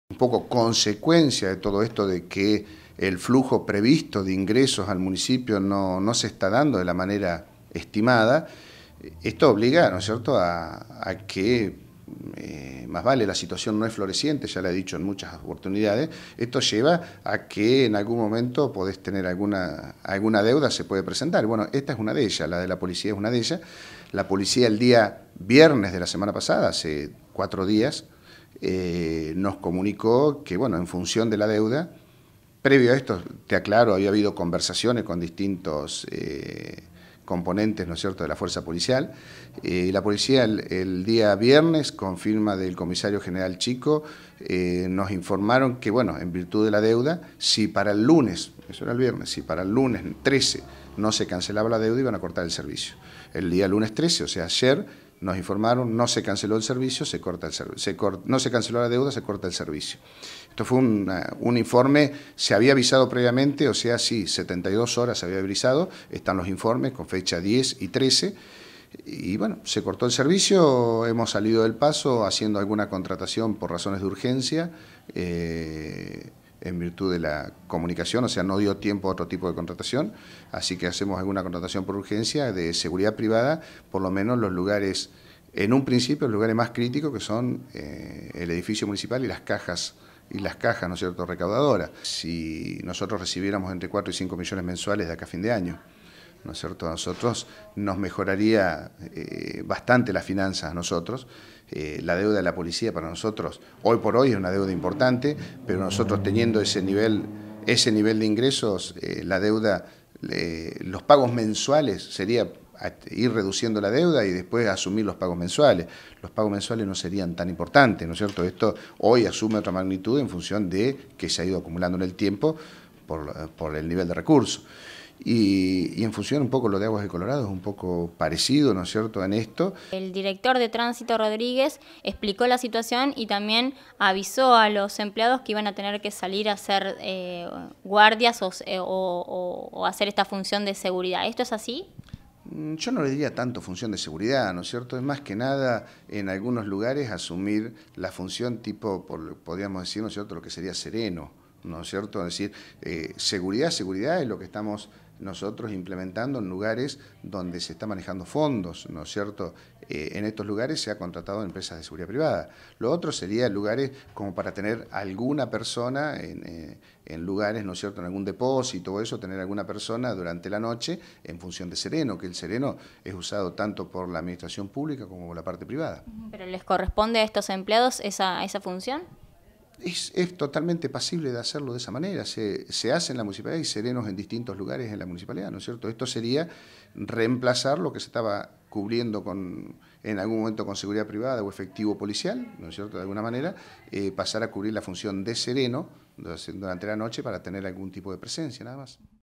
Sin adicionales de policía en edificios municipales
El Ministerio de Gobierno de La Pampa dispuso cesar con los adicionales de la policía que realizan como custodia en edificios municipales. La causa es por una abultada deuda de casi 800 mil pesos. El Secretario de Hacienda del municipio dijo que de manera urgente salieron a contratar seguridad privada.